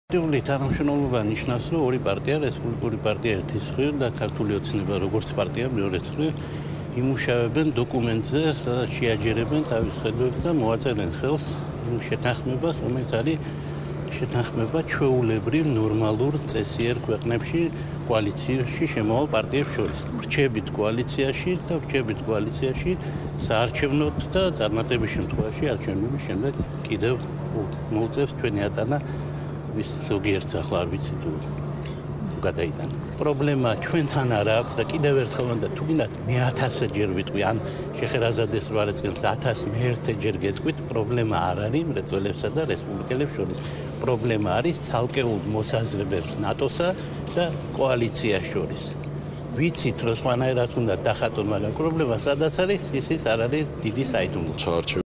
ლევან ბერძენიშვილის კომენტარი ორპარტიულ თანამშრომლობაზე
,,ორპარტიული თანამშრომლობა ნიშნავს, რომ ,,რესპუბლიკური პარტია" ერთი მხრივ და პარტია ,,ქართული ოცნება" მეორე მხრივ, იმუშავებენ დოკუმენტზე, სადაც შეაჯერებენ ხედვებს და მოაწერენ ხელს იმ შეთანხმებას, რომელიც არის შეთანხმება  ჩვეულებრივ, ნორმალურ, წესიერ ქვეყნებში, კოალიციაში შემავალ პარტიებს შორის. სხვა პარტიებსაც შეუძლიათ ჰქონდეთ ასეთი სურვილი, მაგრამ ჩვენ დიდი ხანია ეს სურვილი გამოხატული გვაქვს. გვინდა, რომ ასეთი შეთანხმება გვქონდეს, სადაც ყველაფერი იქნება გაწერილი, ვის რა უფლება აქვს. როგორც პრემიერმა დღეს დაადასტურა, ასეთი საუბარი მართლა იყო. რაც შეეხება სტრატეგიულ შეთანხმებას, ეს არის პარტნიორობის ყველაზე მაღალი ხარისხი. ამიტომ პარლამენტის თავმჯდომარემ ხაზი გაუსვა იმას, რომ გავაძლიერებთ პარტნიორობას და არ შევასუსტებთ. ჩვენ ვრჩებით კოალიციაში საარჩევნოდ და წარმატების შემთხვევაში კიდევ მოუწევთ ჩვენი ატანა ზოგიერთს", - ლევან ბერძენიშვილი.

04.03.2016